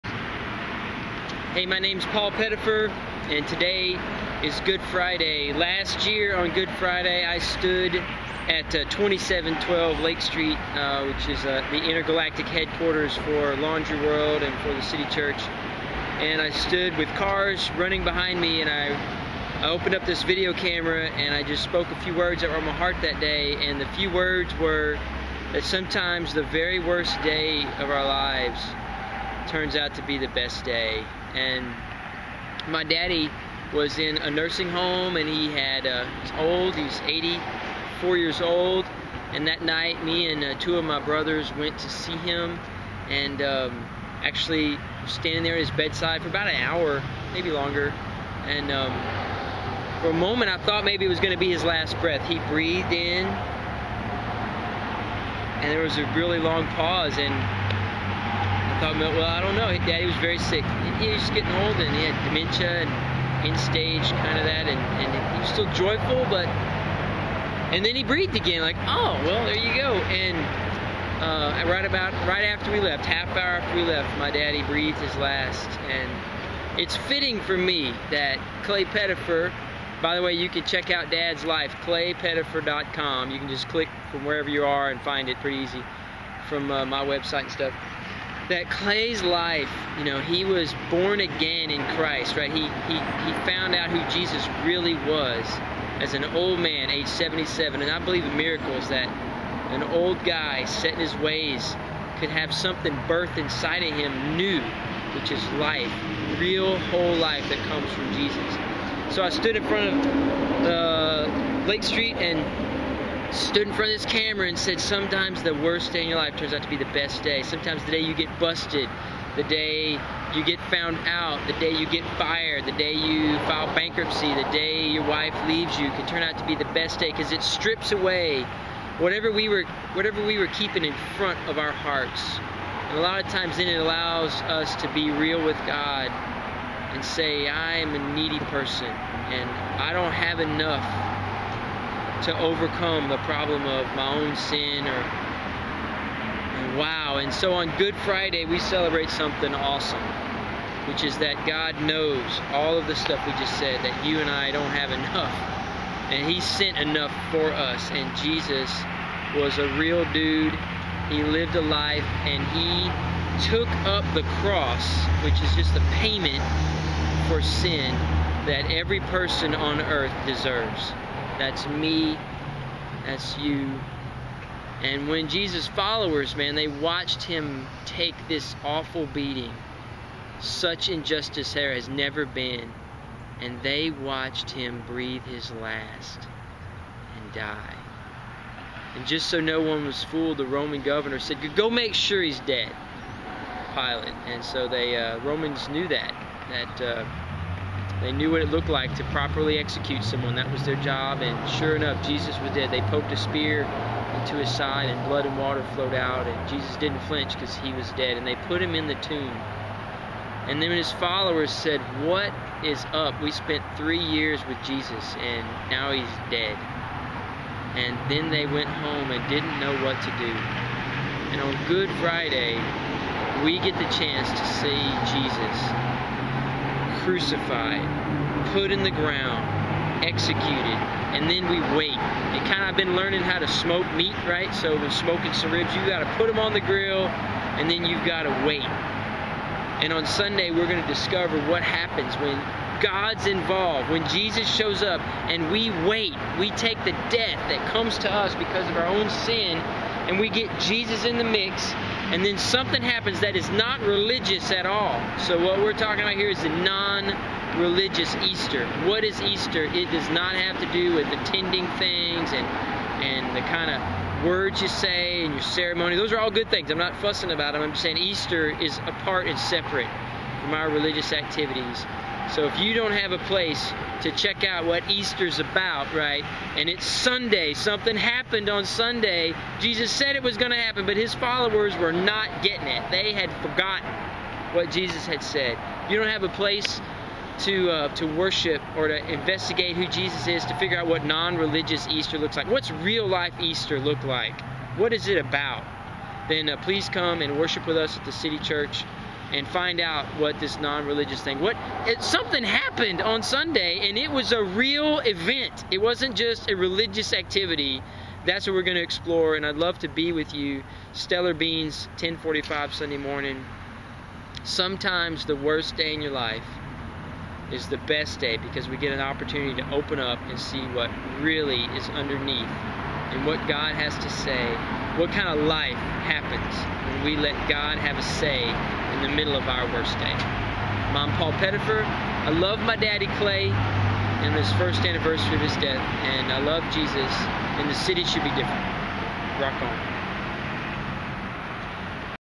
Good Friday 2012, 1 yr after my dad died - Death precedes Resurrection
Last year on Good Friday, I recorded this video, about how sometimes the worst day of your life turns out to be the best day.  That night, my awesome dad died When it's Friday (the day Jesus was executed for everyone else's crimes), we can't quit...you have to look for the Resurrection that's coming!  This is why the Non-Religious Easter matters...Jesus is no longer dead, he rose from the dead - Easter!